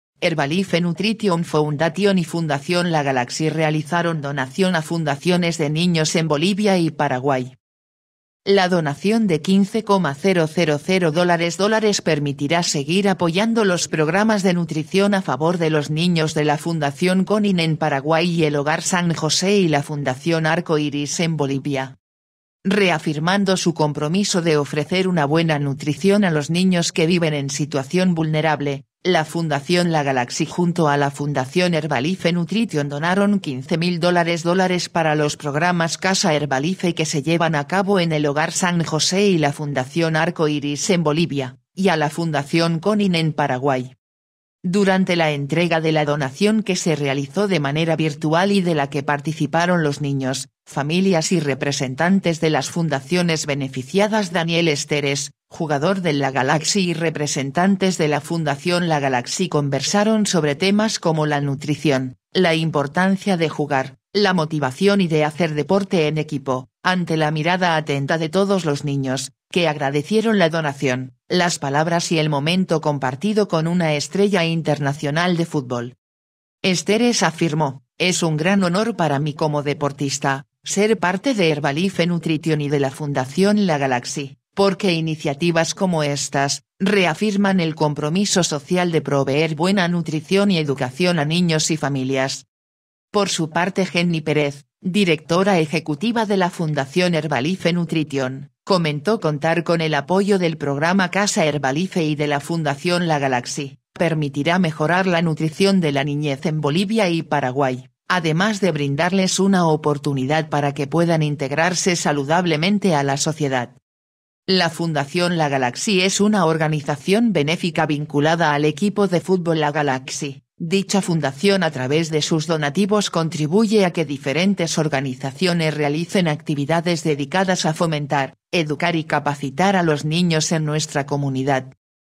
Herbalife Nutrition Foundation y Fund LA Galaxy realizaron donación a fundaciones d niños en Bolivia
• La donación de $15,000 dólares permitirá seguir apoyando los programas de nutrición a favor de los niños de la Fundación Conin en Paraguay y el Hogar San José y la Fundación Arco Iris en Bolivia.